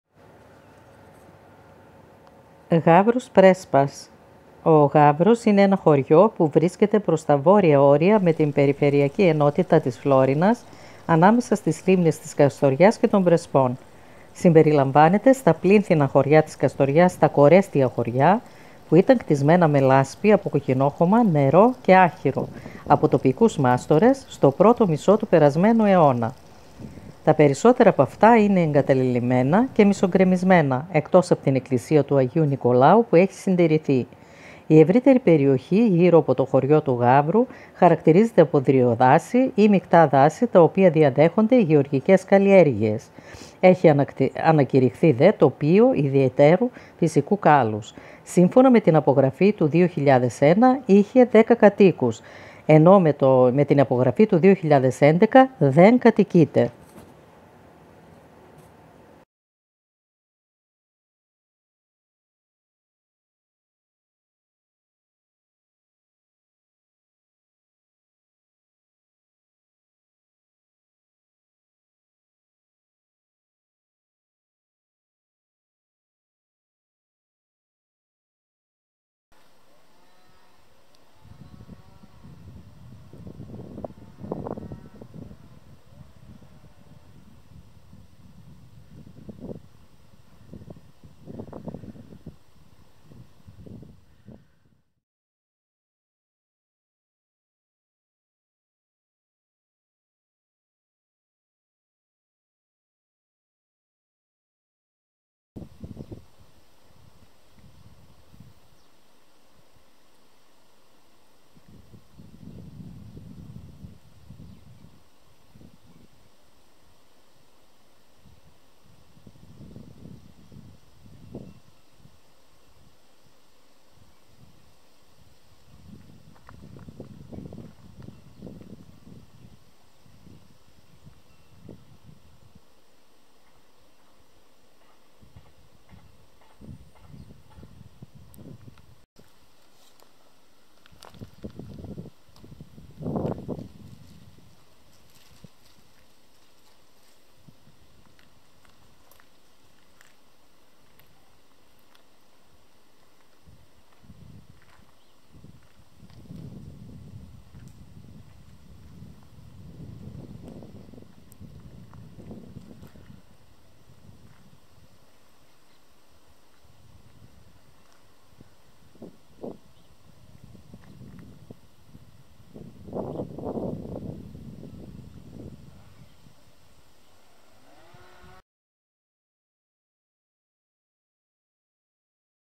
Gavros Florinas-Γαύρος Φλώρινας Greece anna traveler
It is a village to the northern borders with the peripheral unit of Florina Greece It is included in the brick villages of Kastoria in Korestia villages that were built with mud from red soil, water and straw by local craftsmen in the first half of the last century Most of these houses are abandoned and half-destroyed except for the church of Agios Nikolaos which has been preserved.

anna traveler